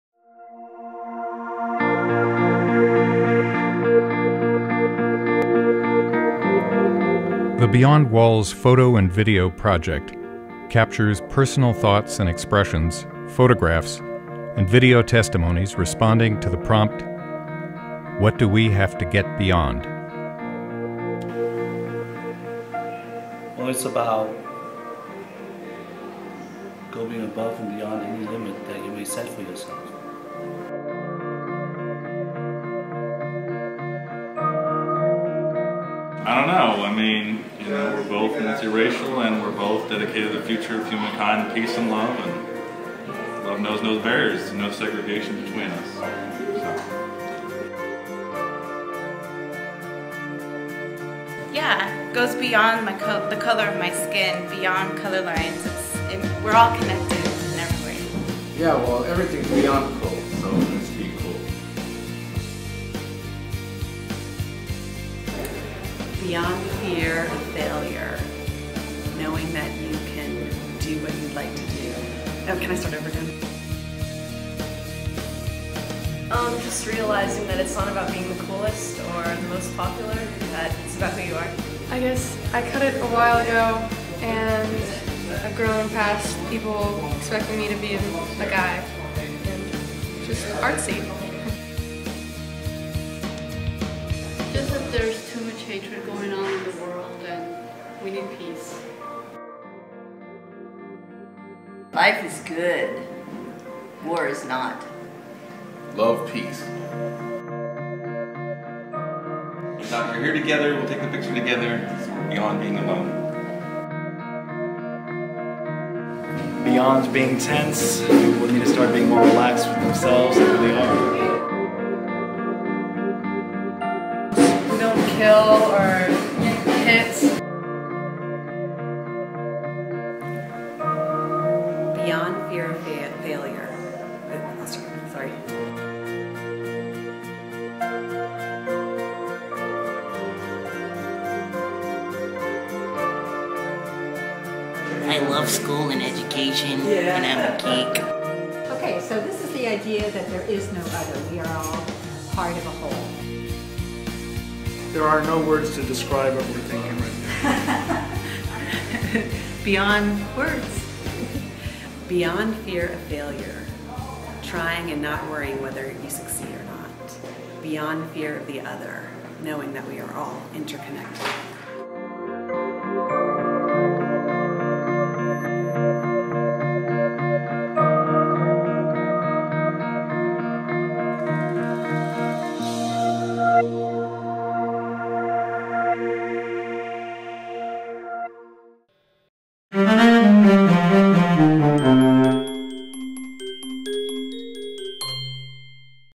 Beyond Walls Photo and Video Project
The BEYOND WALLS Photo and Video Project captured personal thoughts and expression, photographs, and video testimonies responding to the prompt: What do we have to get BEYOND?

The first participants posted responses that included moving "Beyond prejudice," "Beyond judgement," "Beyond convention," even "Beyond cool!"  We've posted these for viewing on the BEYOND WALLS Photo and Video Project Facebook page at Facebook.com/BeyondWallsProject.

Now it's your turn. Participate in the worldwide BEYOND WALLS Photo and Video Project!

What do we have to get BEYOND?

Let the world know.

Take your picture! Video your thoughts! Post your response to the BEYOND WALLS Photo and Video Project at Facebook.com/BeyondWallsProject.

Spread the word to your networks.

Start to change the world.

Start to move. BEYOND WALLS!